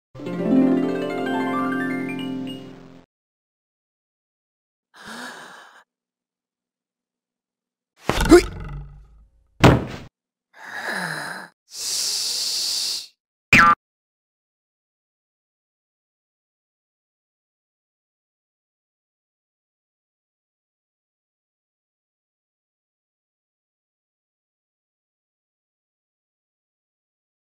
Doodle Danger - Cupid's Arrow
Love is in the air for the doodles.  And it hurts.  Have a happy Valentine's Day everyone.